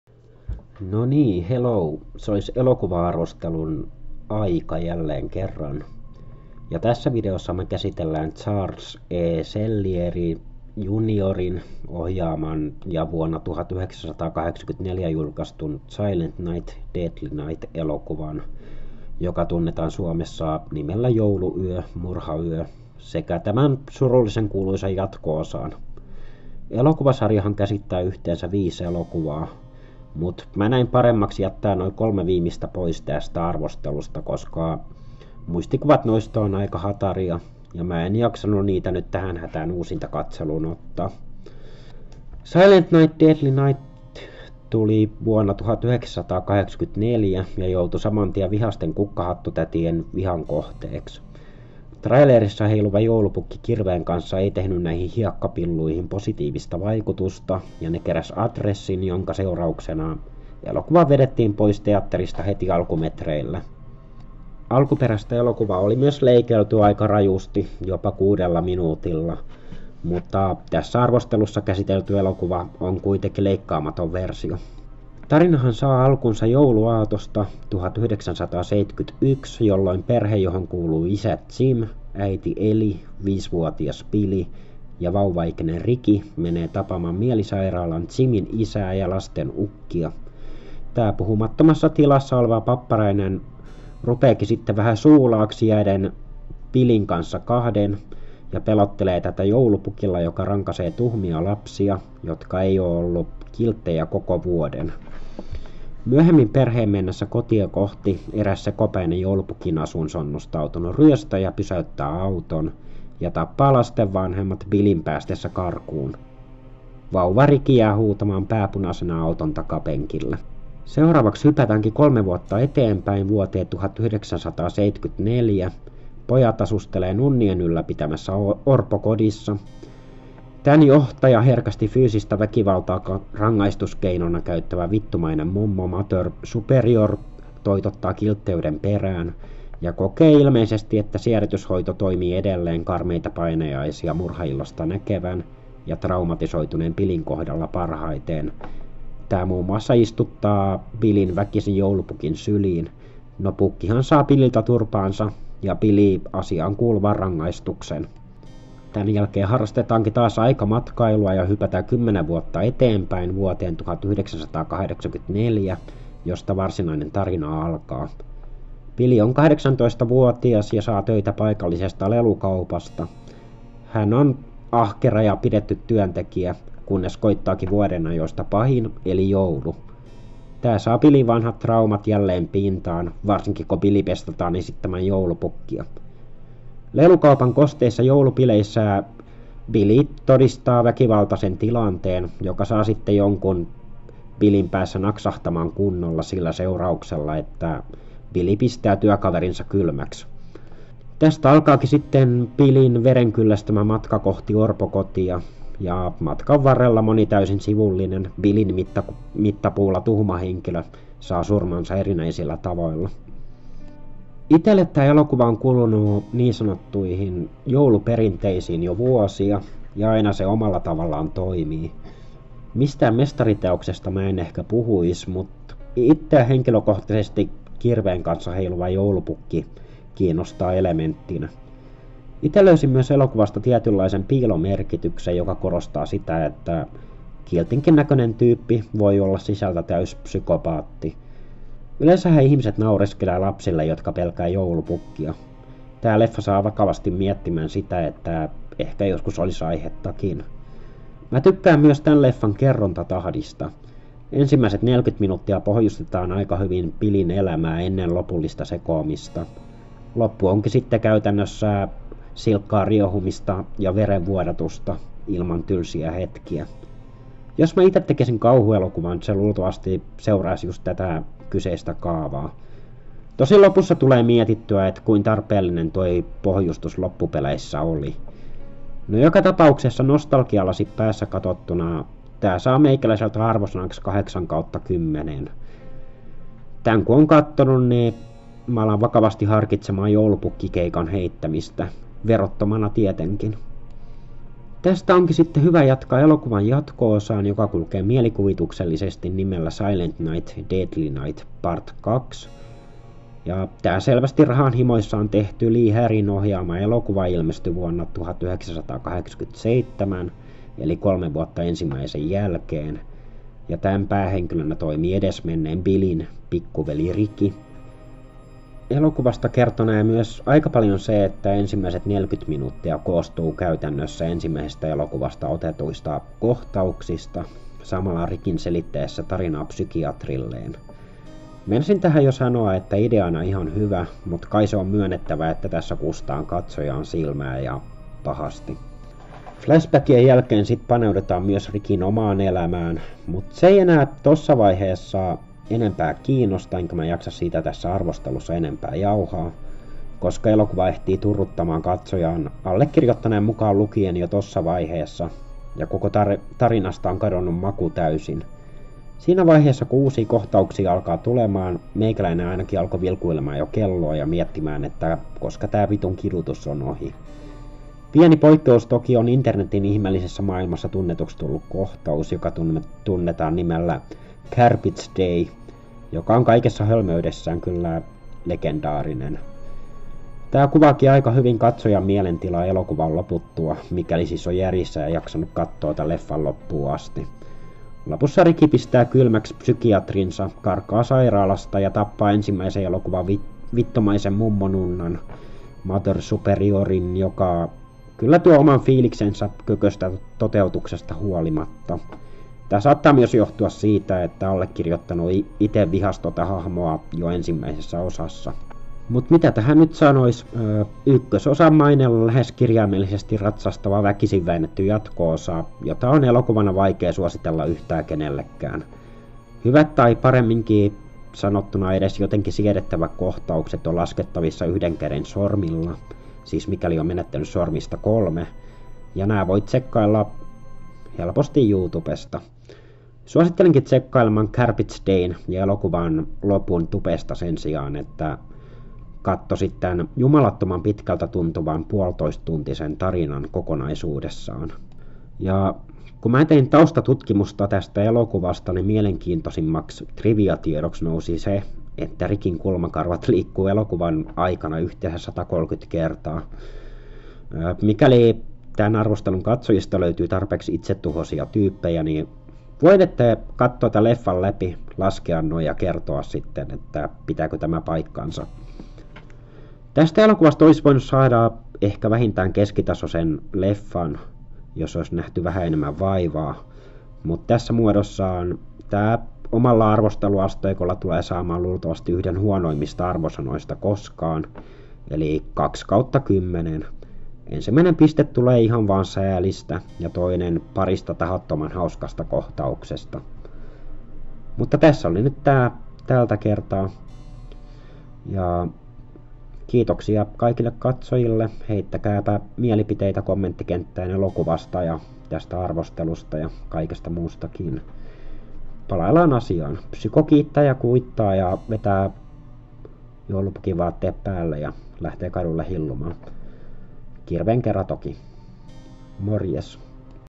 Elokuva-arvostelu: Silent Night, Deadly Night (1984) & SNDN Part 2 (1987)
Arvostelussa vuoden 1984 Silent Night, Deadly Night / Jouluyö, murhayö ja tämän 1987 vuoden jatko-osa Silent, Night Deadly Night Part 2